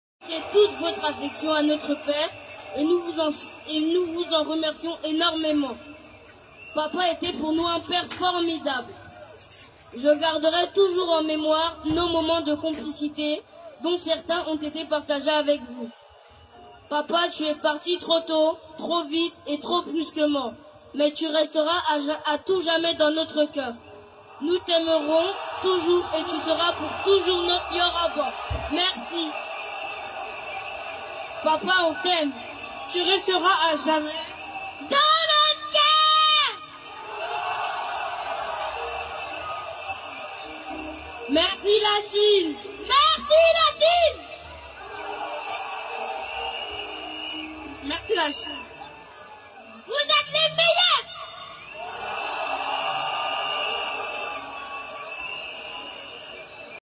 LES ENFANTS DE DJ ARAFAT LUI REND UN HOMMAGE TOUCHANT
LES ENFANTS DE  DJ ARAFAT LUI PARLE